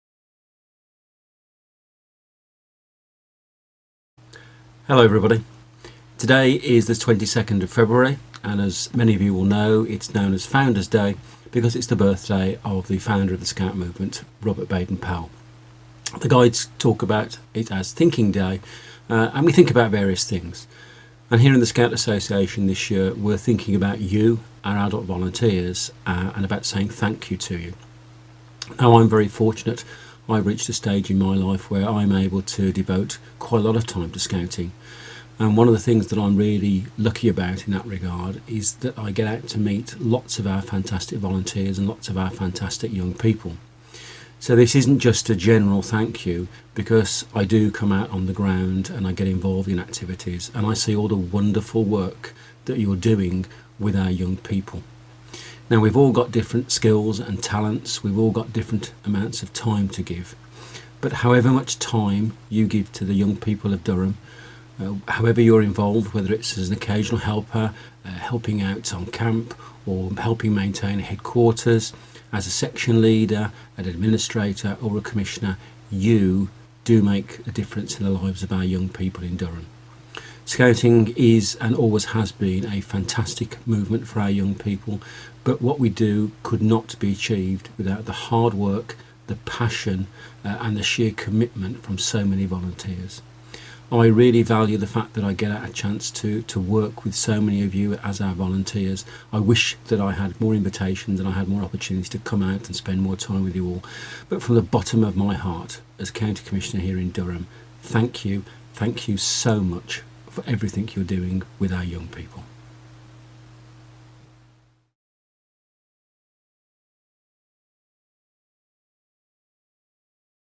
CC's Blog - Founders Day 2019 (Thank You)
My personal thank you video, thanking the 2,500+ volunteers in Durham Scouts, on Founder's Day 2019